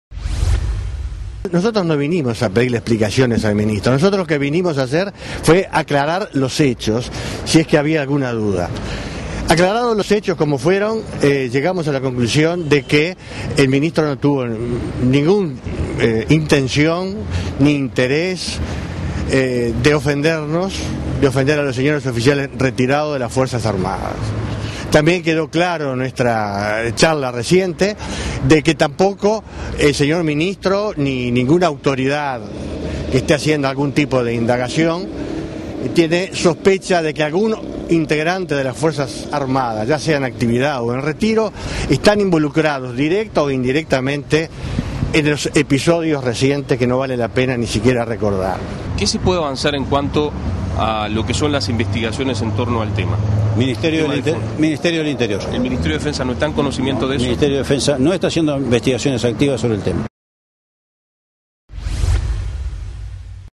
¿Rosadilla pidió perdón? Hoy los militares afirman que el ministro no tuvo interés de ofenderlos
Círculos de militares retirados y en actividad enviaron ayer una carta al ministro Luis Rosadilla reclamando que rectifique sus declaraciones y sugiriendo que pida perdón por éstas. Hoy se reunieron con el ministro Rosadilla y tras el encuentro el Almirante Carlos Maglioca dijo que el ministro "no tuvo ningún interés de ofender a los oficiales retirados". Aseguró que "quedó claro que  el ministro, ni ninguna autoridad sospechan que algún integrante de las FF.AA estuviera involucrado en los episodios recientes.